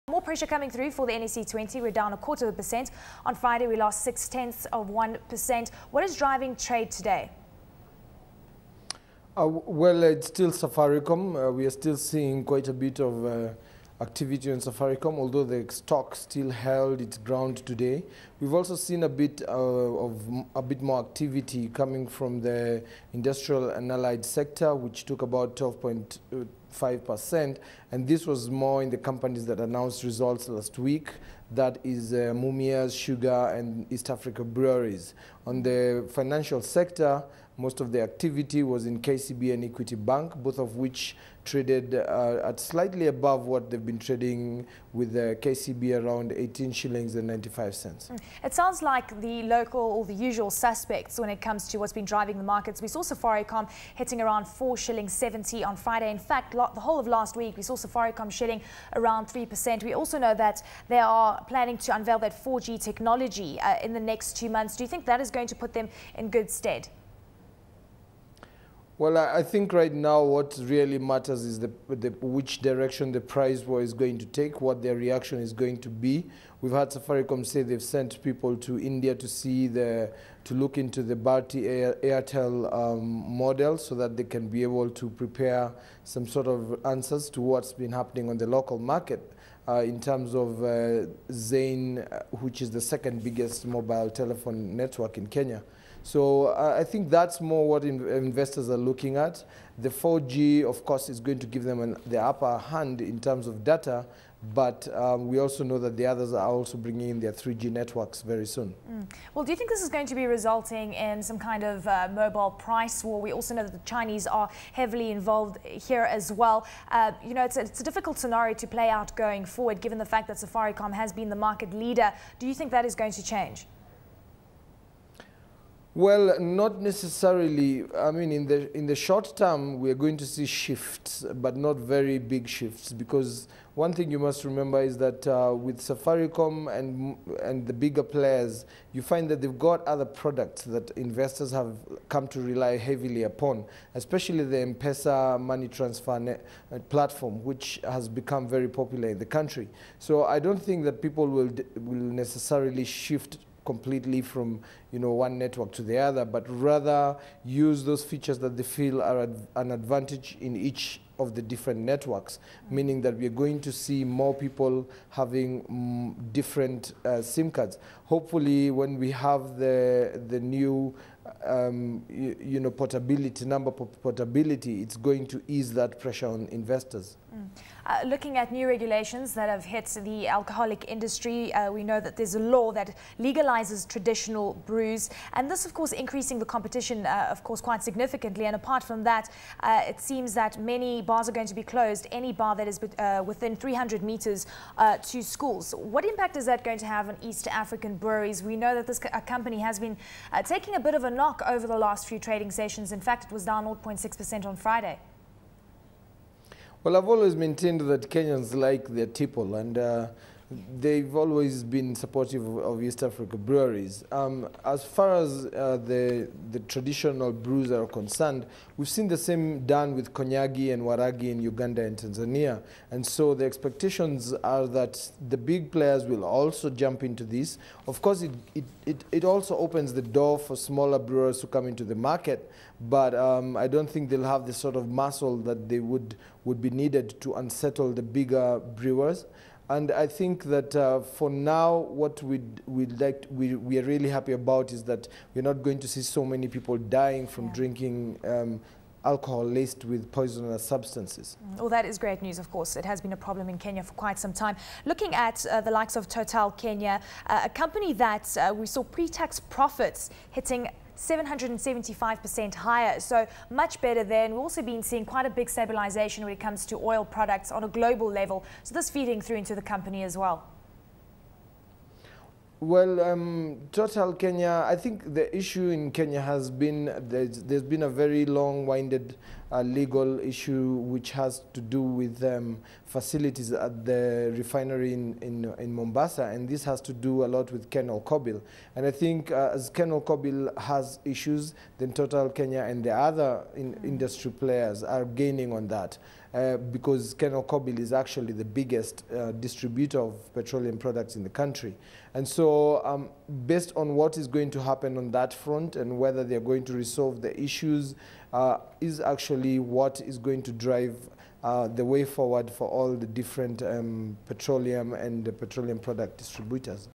6 September - Kenyan Markets Wrap - Samuel Gichohi - Standard Investment Bank
Eleni Giokos speaks with Samuel Gichohi, Investment Analyst at Standard Investment Bank, looking at: Main drivers on the market; Safaricom plans with 4G; New alcohol regulations; Total Kenya numbers.